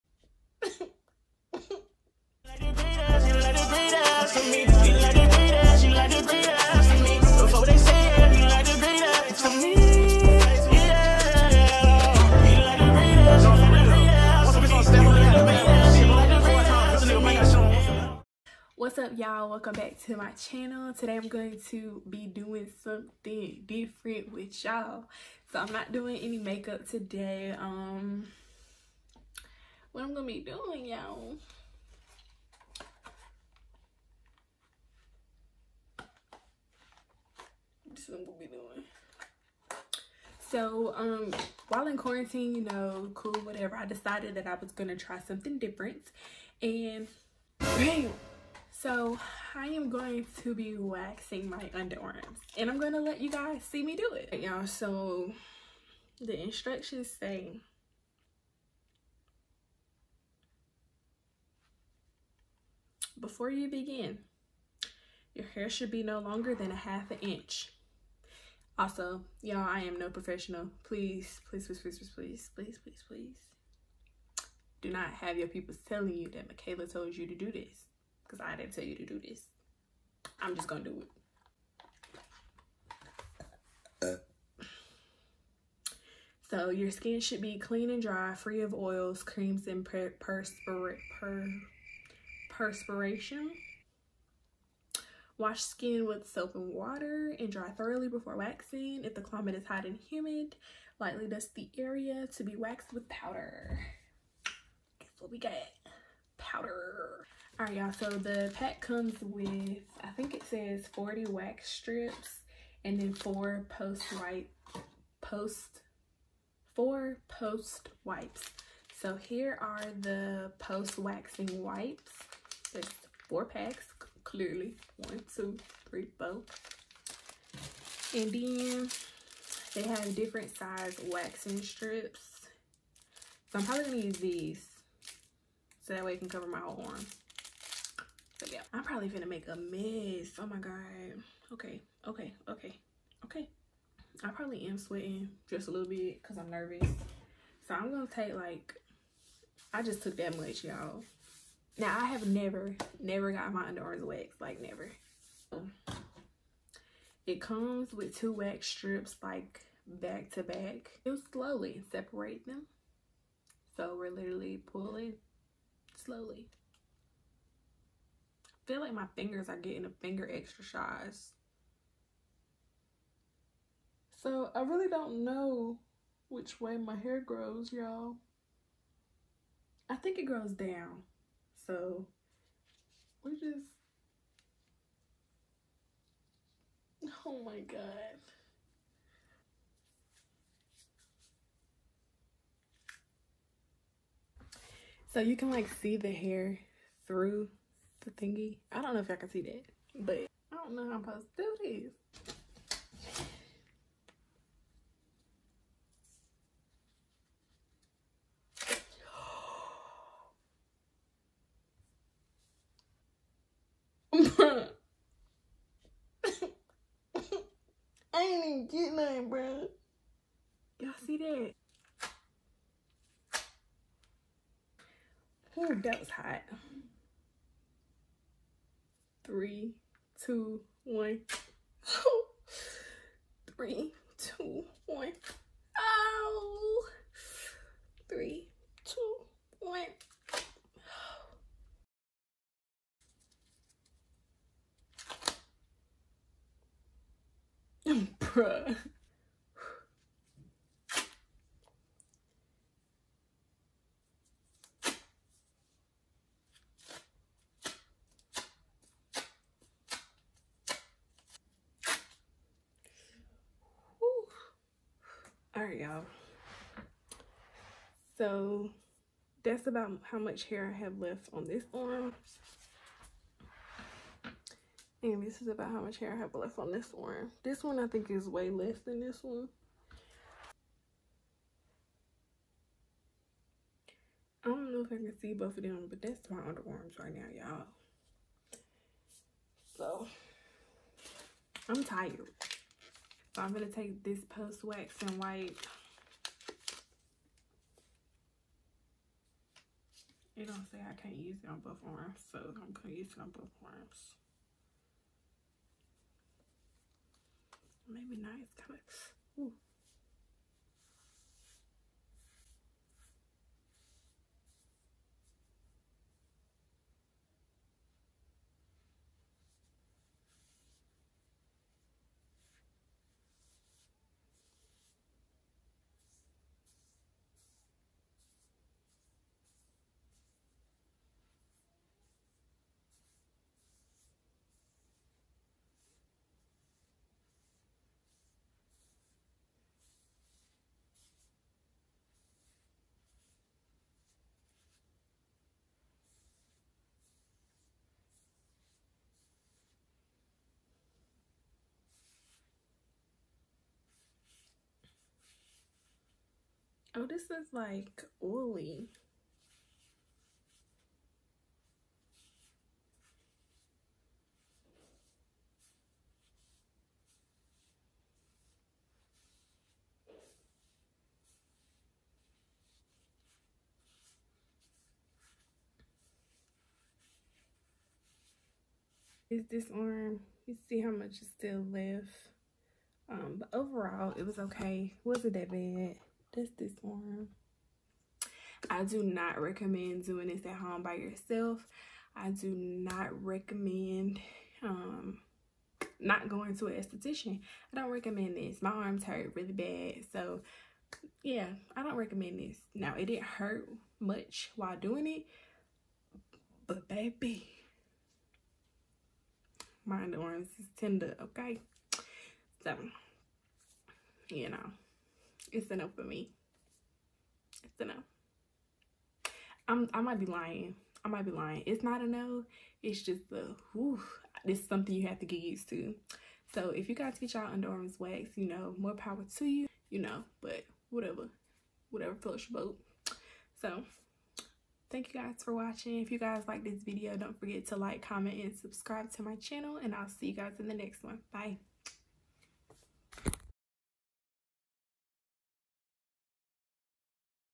Y’all I tried to wax my underarms.... 🤦🏽‍♀️ | Quarantine boredom
Please don’t be like me guys, make sure you follow all instructions on the package. If you’ve never done this before, i recommend seeing an esthetician before performing on yourself.